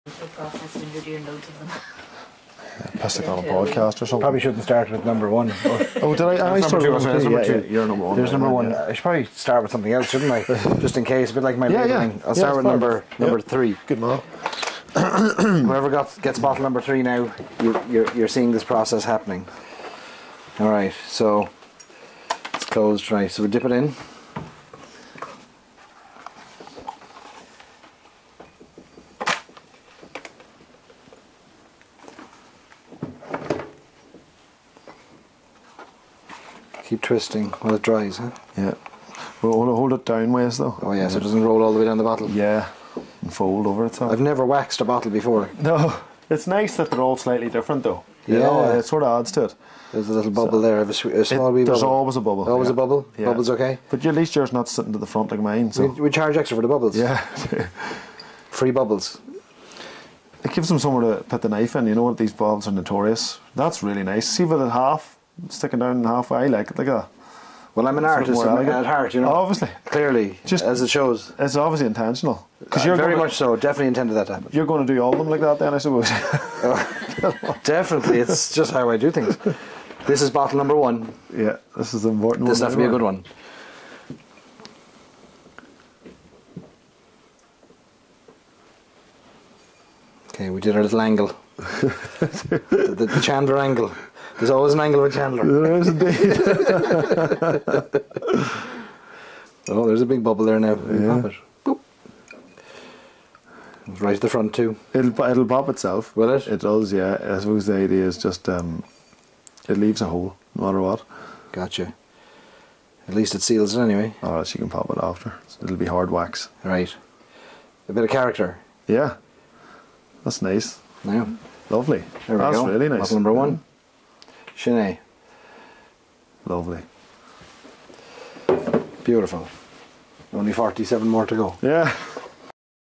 Wax-dipping our Killowen Distillery Barántúil bottles
Makers Mark with all its fancy wax cookers and gallons of wax is no match for a counter-top kitchen slow-cooker in the mountains of Mourne at Killowen Distillery.

Not long to go until you can have your very own non-uniformly wax-dipped bottle of our Stories & Sips x Killowen Distillery Barántúil whiskey in your hands.

Available to our Whiskey Club members anywhere in the world tomorrow, Wednesday, at 11am EST/4pm Irish time

It's the first whiskey from Killowen Distillery to ever reach 4 years of age and we're delighted with how it has matured.

Sláinte.